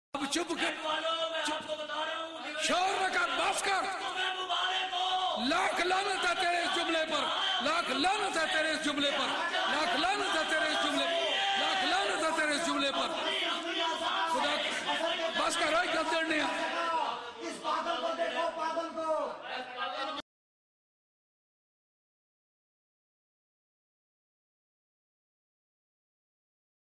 Abdul Qadir Shah Abusing
THE FOLLOWERS OF ABDUL QADIR SHAH USE THE LABEL "MUFAKKIR E ISLAM" WITH HIS NAME. BUT HE ABUSES ULAMA E AHL E SUNNAT. HOW CAN HE BE A MUFAKKIR E ISLAM? ABUSING IS FISQ IN ISLAM AND ABUSING AN AALIM DUE TO HIS KHOWLEDGE IS KUFR.